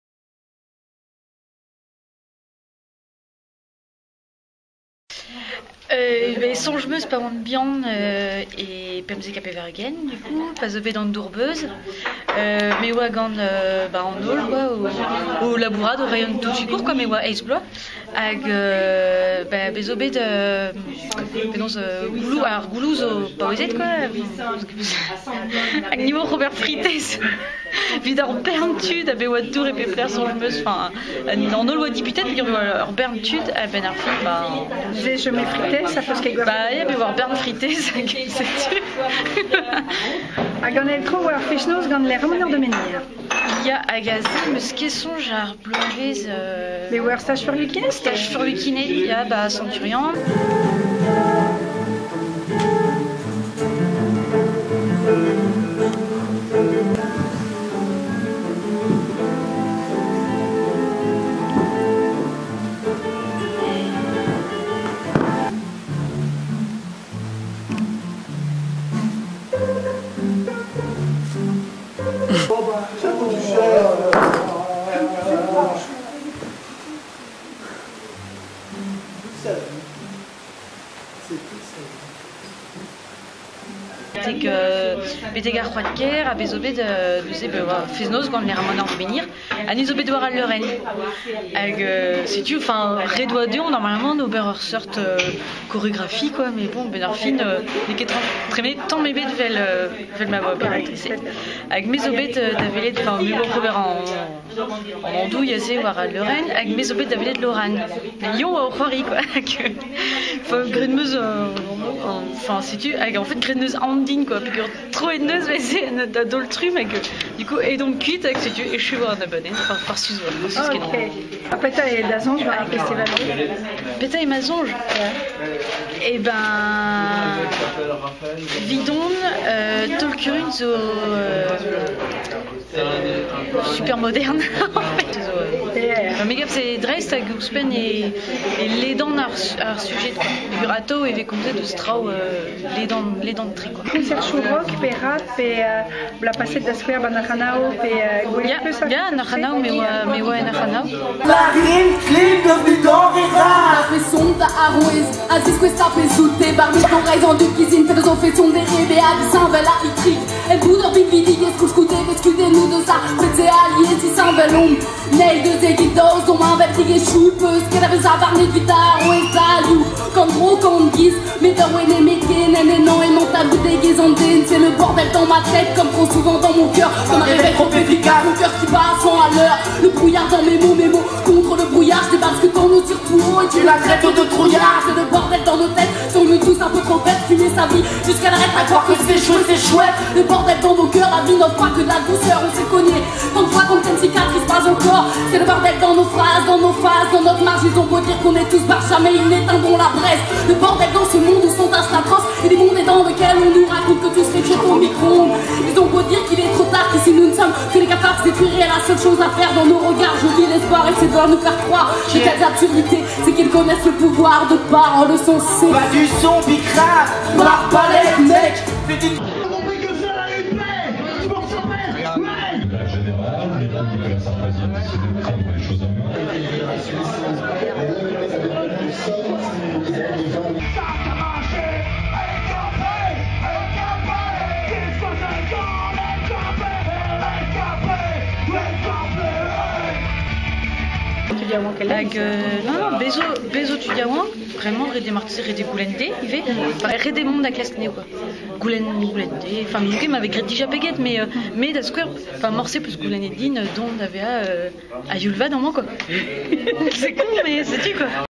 Taol Kurun Lenig : fritez, furlukin hag abadennoù rock
Pignet war al leurenn e 1995, gwerzhet fritez. Ha distroet koshoc'h er vro, staj furlukin hag abadenn rock b'an Arzhanaou